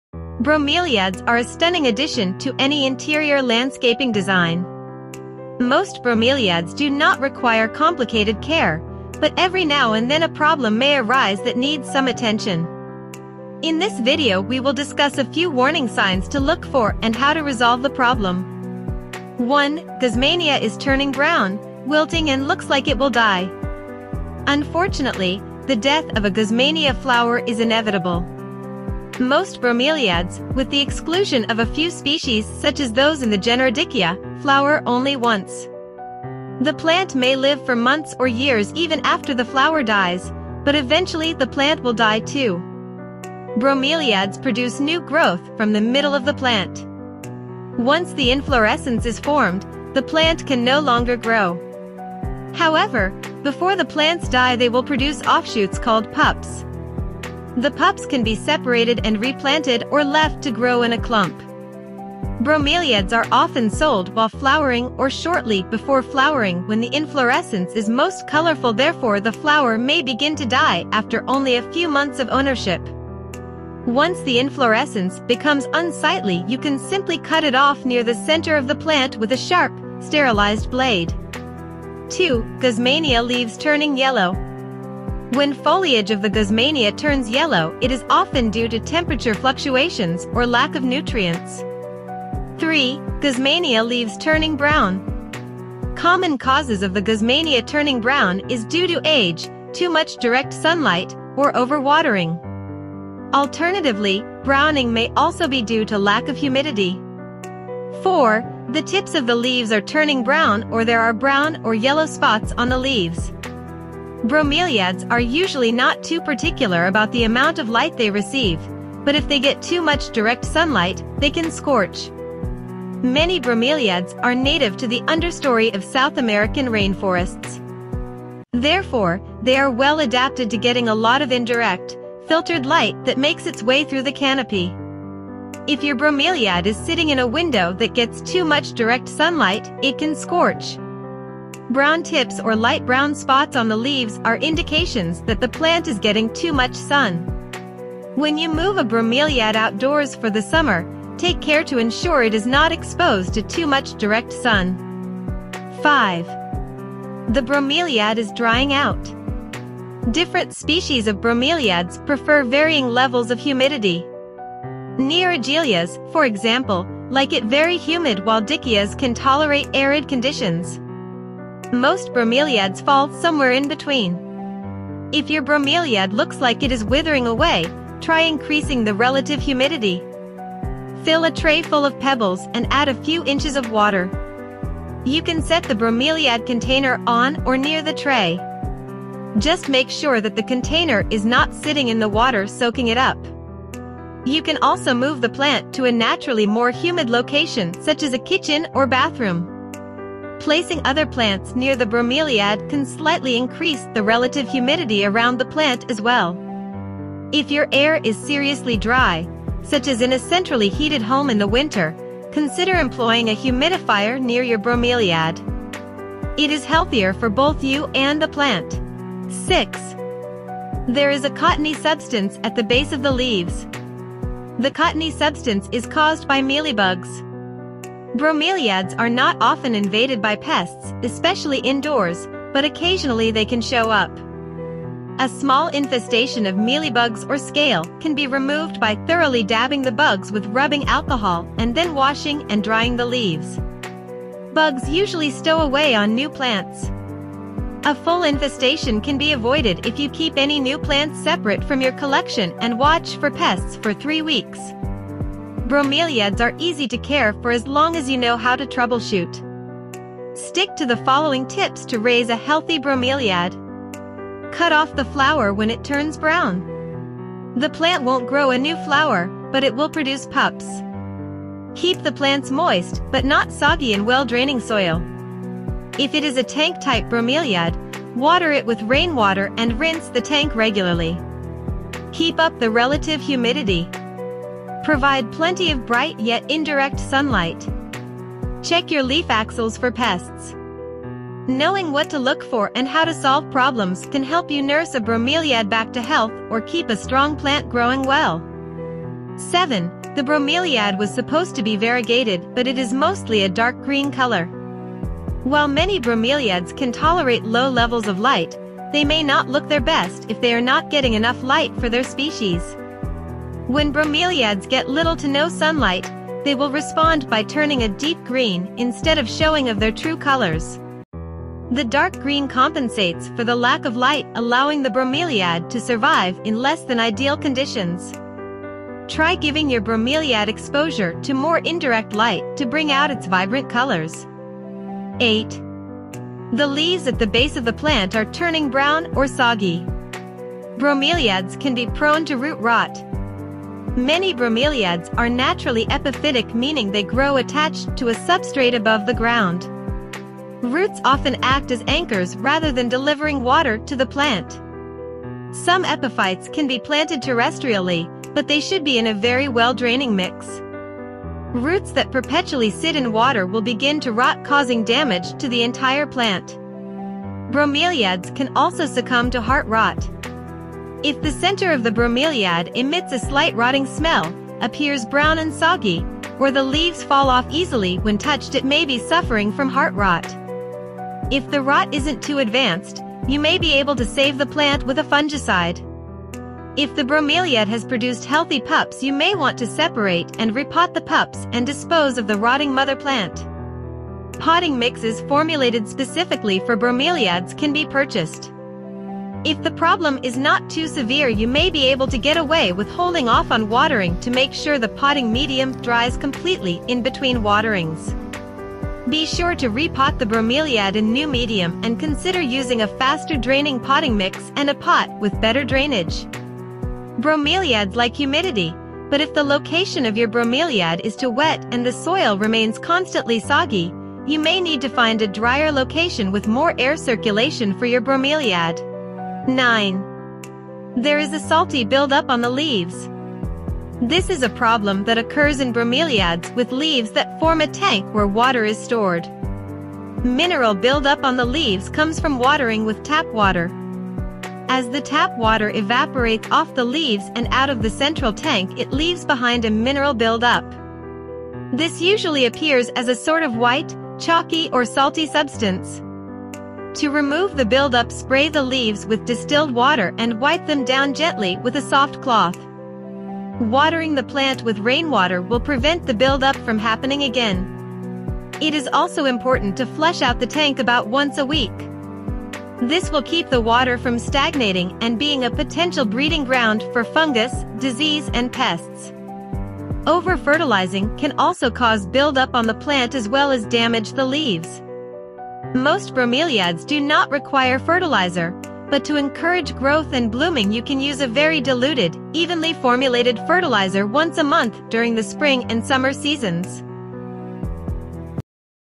Common Problems With Guzmania Bromeliad: Guzmania Leaves Turning Yellow or Brown!
Common Problems With Guzmania Bromeliad:
Bromeliads are a stunning addition to any interior landscaping design. Most bromeliads do not require complicated care, but every now and then a problem may arise that needs some attention. In this video we will discuss a few warning signs to look for and how to resolve the problem.
0:22 Guzmania Bromeliad is turning brown, wilting and looks like it will die.
1:22 Guzmania Bromeliad Leaves Turning Yellow
1:32 Guzmania Bromeliad Leaves Turning Brown
1:47 The tips of the leaves are turning brown or there are brown or yellow spots on the leaves.
2:37 The Guzmania Bromeliad is drying out.
3:40 There is a cottony substance at the base of the leaves.
5:00 The Guzmania Bromeliad was supposed to be variegated but it is mostly a dark green color.
5:35 The leaves at the base of the plant are turning brown or soggy.
7:10 There is a salty build up on the leaves.